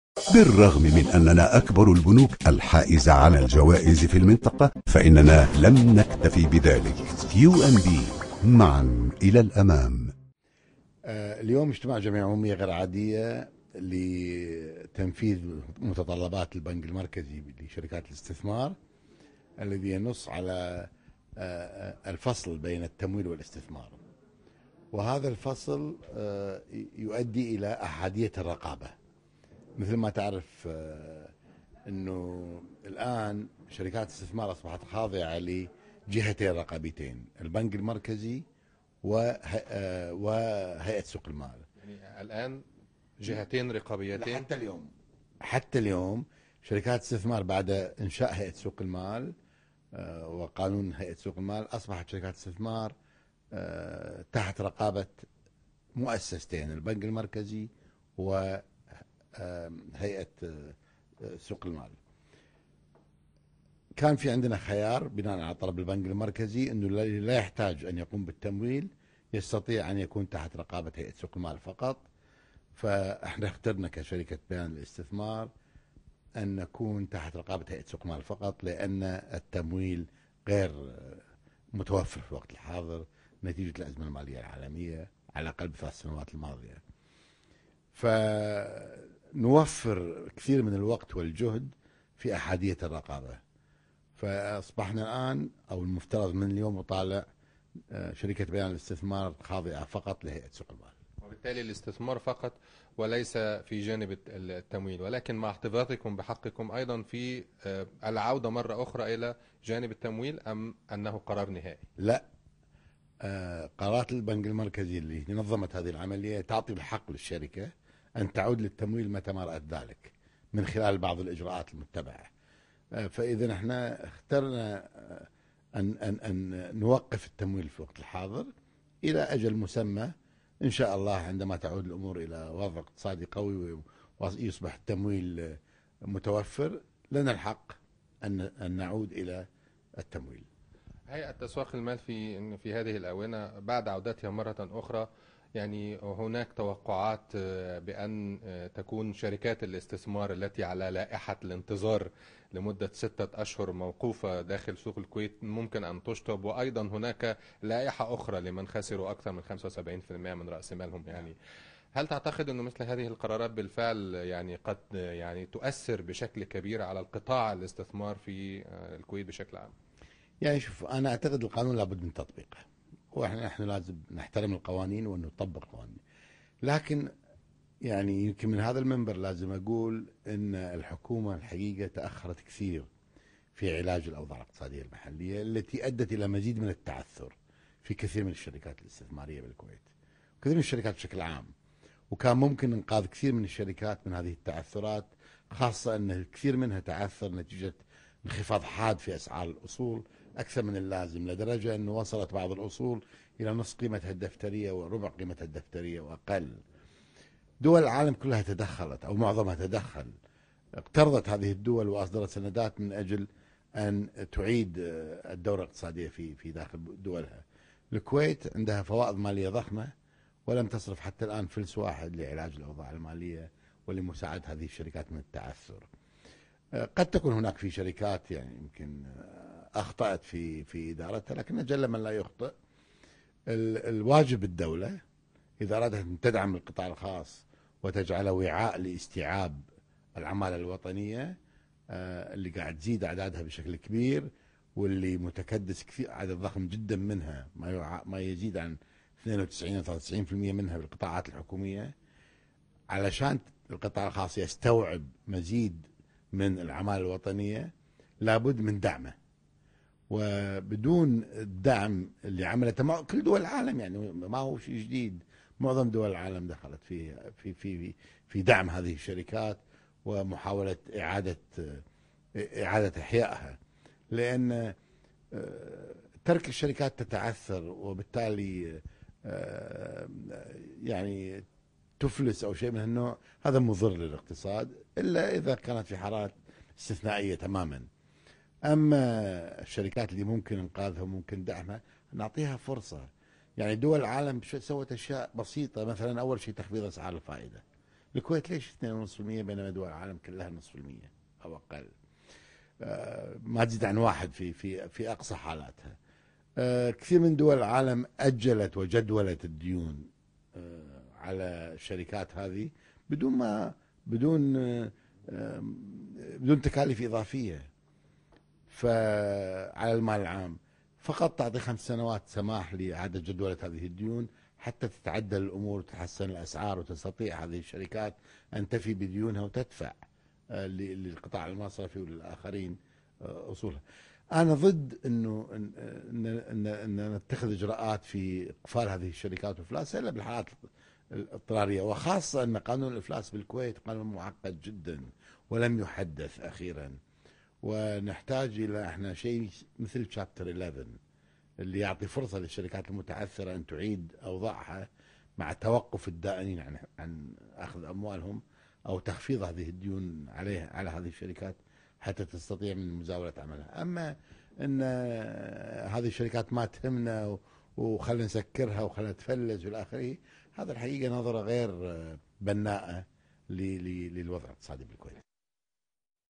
بيان للاستثمار تفصل النشاط التمويلي وتكتفي بالاستثمار
Bayan Investment gives up the financing activity and keeps the investing.